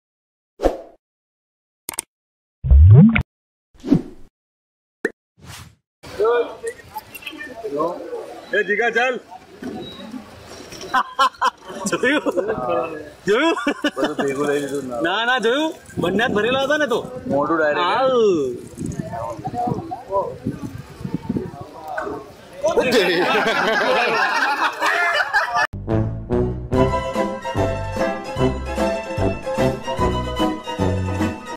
Curb Your Monkey Tricks
The monkey slaps the person for not giving his biscuit. Watch to find out more on the topic.